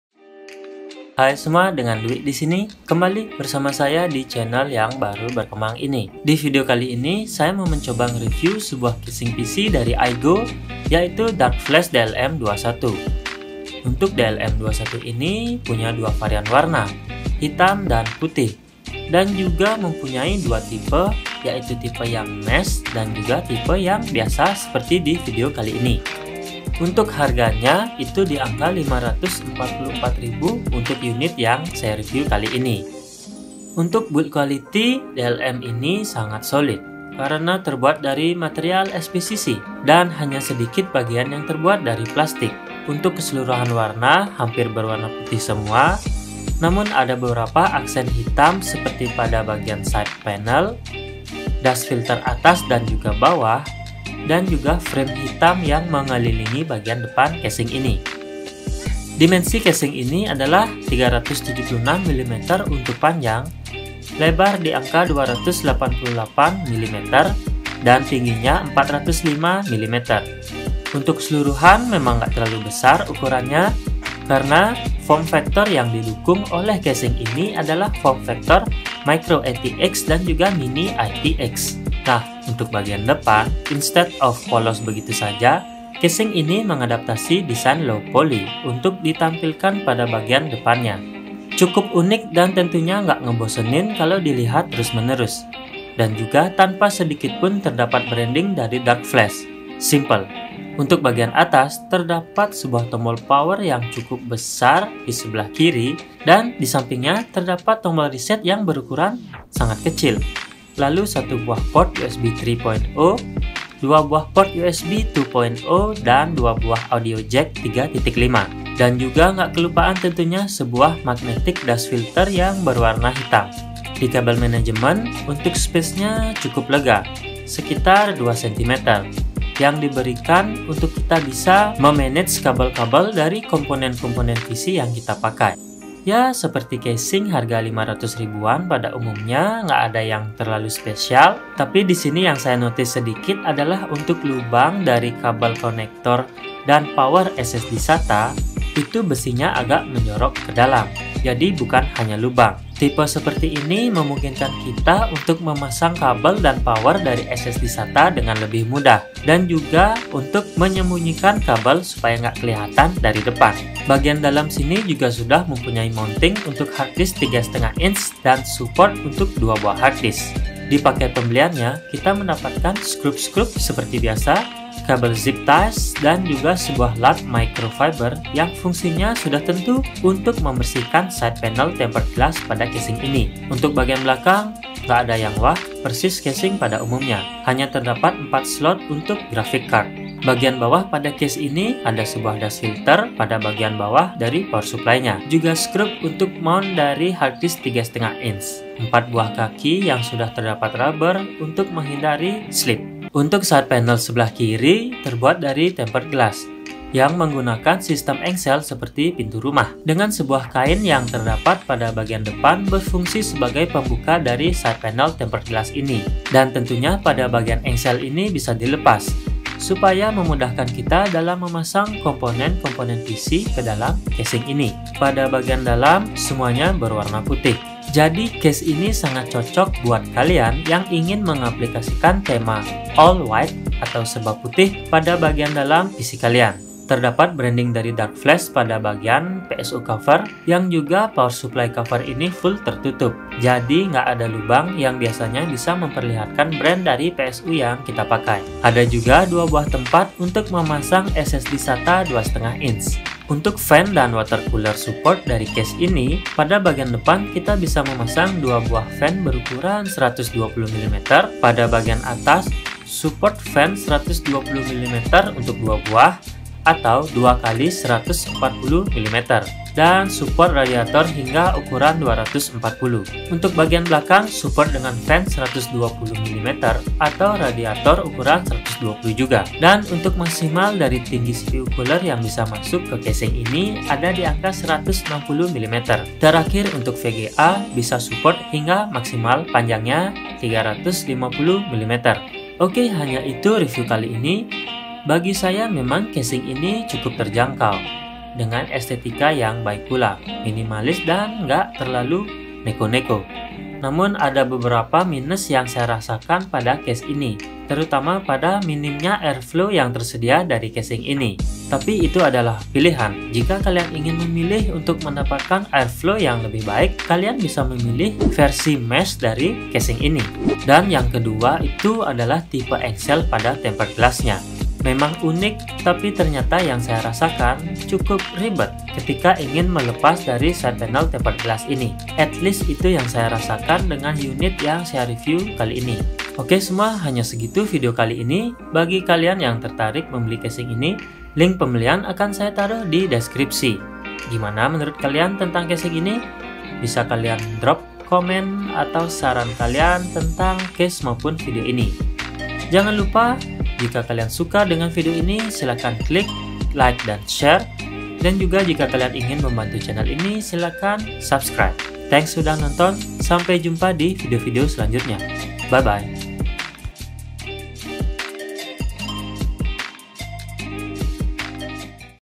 Review Aigo Darklash DLM21 White Casing Minimalist Harga Terjangkau
Hi semua, Kali ini saya mau coba Review casing sedikit yaitu Aigo Darkflash DLM21 (Non mesh)